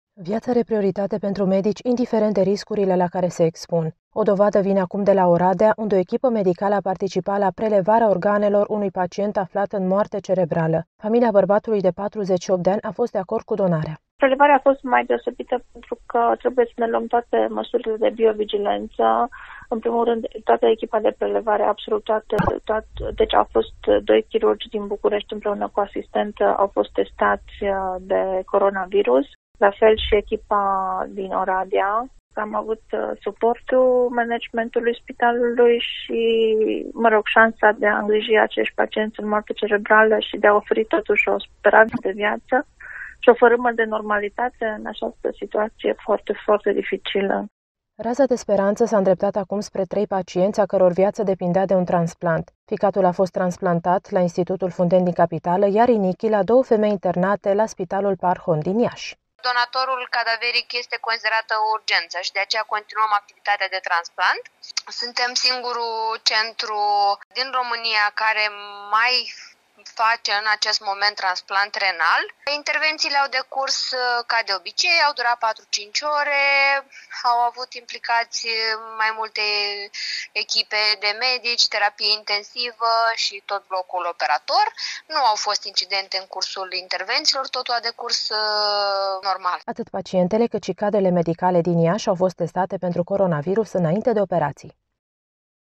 În plină criză la Oradea s-a reuşit prelevarea organelor unui pacient aflat în moarte cerebrală
Viaţa are prioritate pentru medici, indiferent de riscurile la care se expun. O dovadă vine acum de la Oradea, unde o echipă medicală a participat la prelevarea organelor unui pacient aflat în moarte cerebrală. Familia bărbatului de 48 de ani a fost de acord cu donarea.